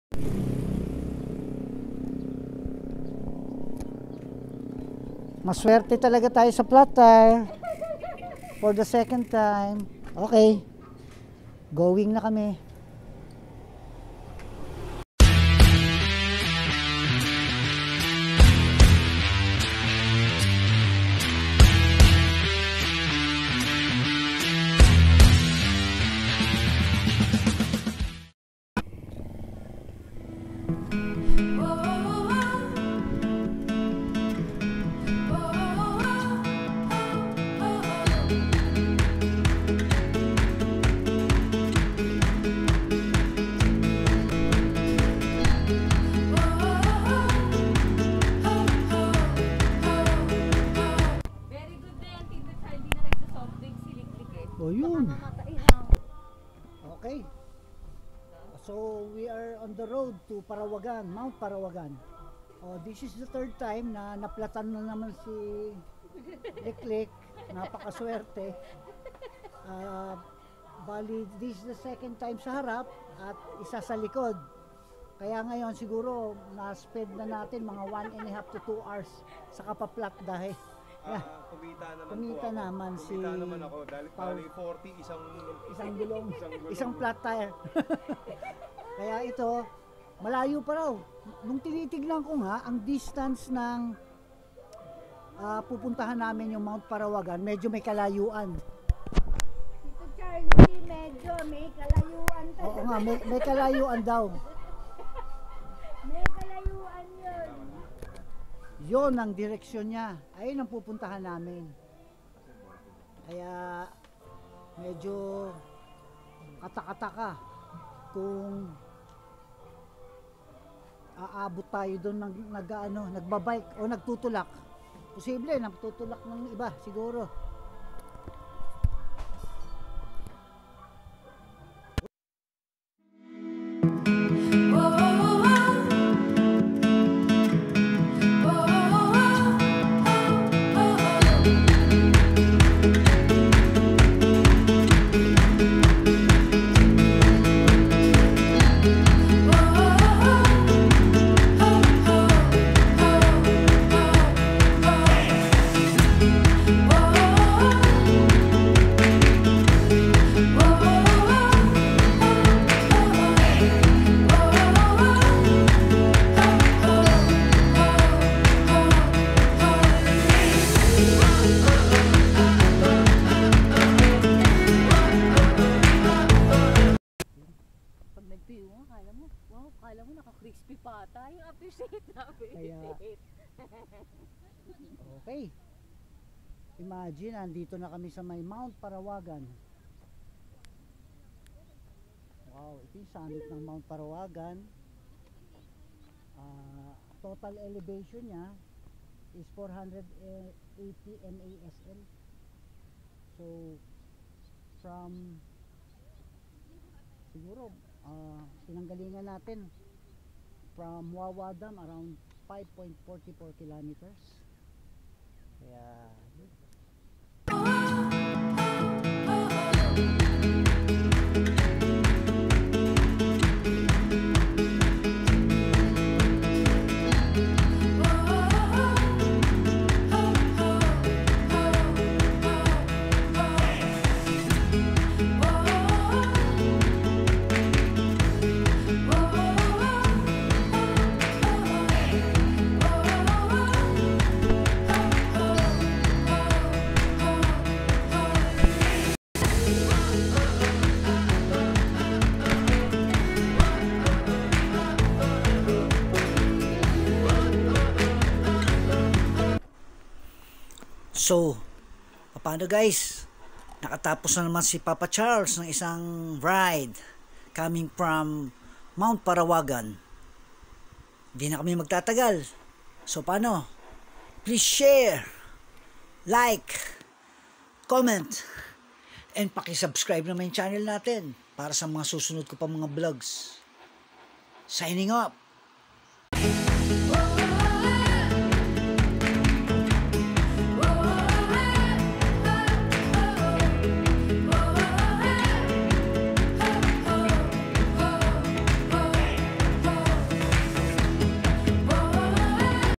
NAPADPAD SA MOUNT PARAWAGAN!
This is a regular Sunday ride with Paolo, Angelique, Madel of family bikers. We planned to conquer Mount Ayaas Montalban but ended up in Mount Parawagan due to health personnel's regulations in Montalban...